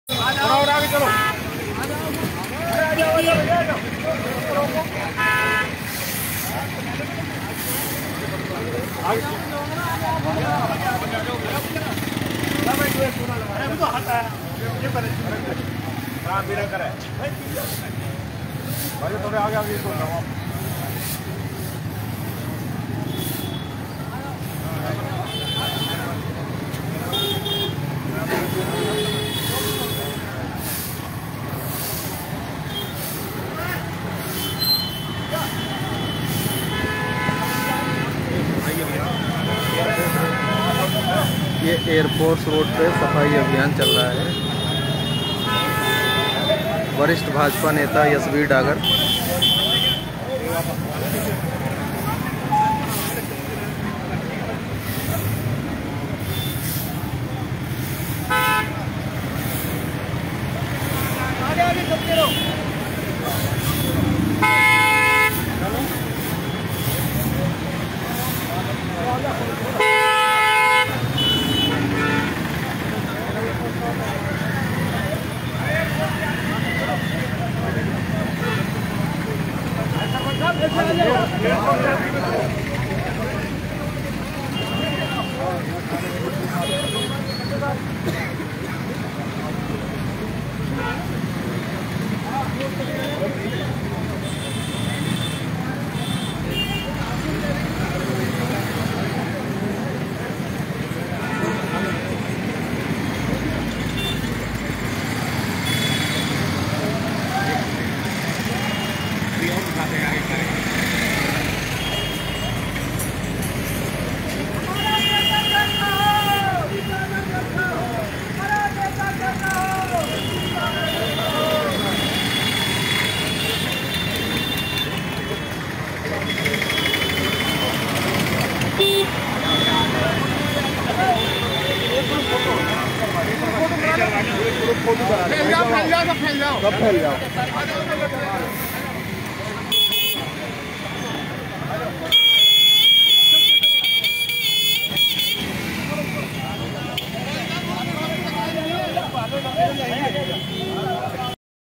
वरिष्ठ भाजपा नेता यशवीर डागर.... झाड़ू लेकर साफ सफाई करते दिखे ..
वरिष्ठ भाजपा नेता यशवीर डागर.
झाड़ू लेकर साफ सफाई करते दिखे ..